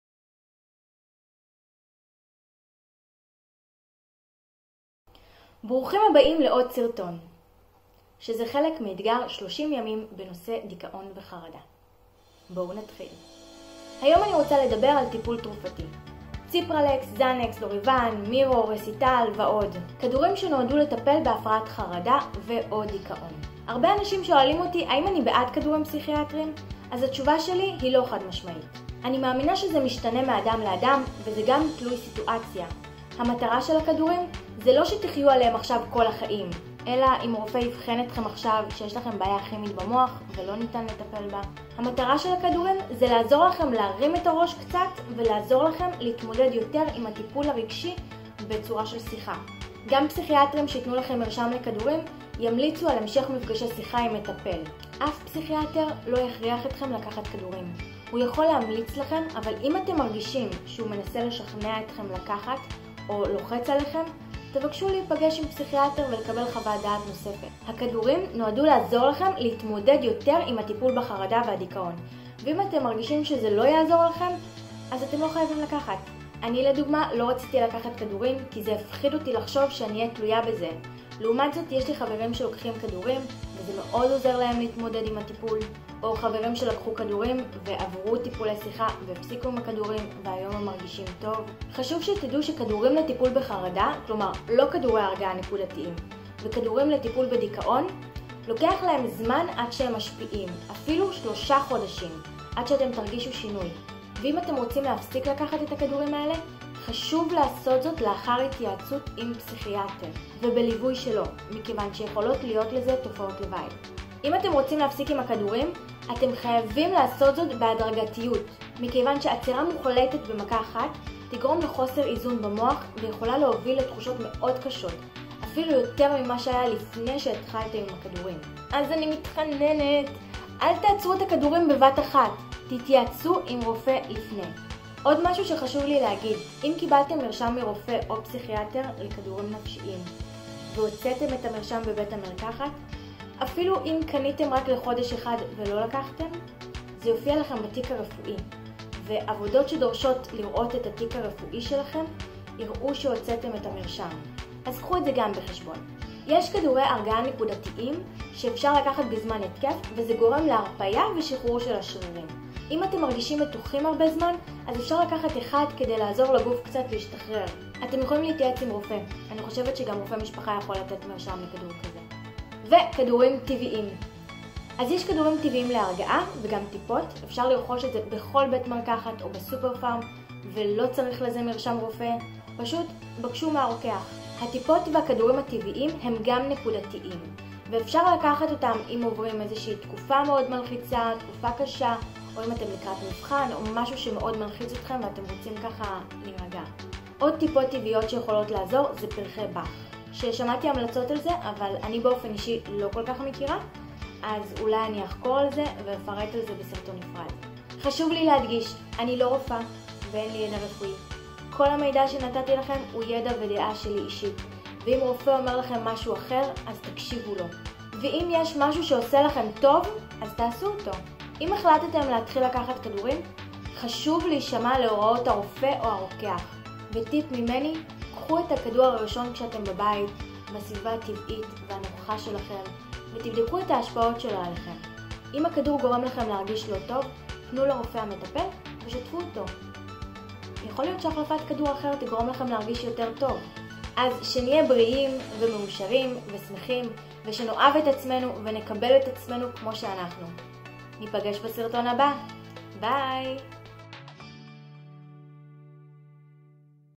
כל האמת על טיפול תרופתי בדיכאון וחרדה וכדורים פסיכיאטרים!!! האם כדאי לקחת כדורים פסיכיאטריים???
גם אתם יכולים לנצח את החרדה והדיכאון, בהצלחה!
בר ❤️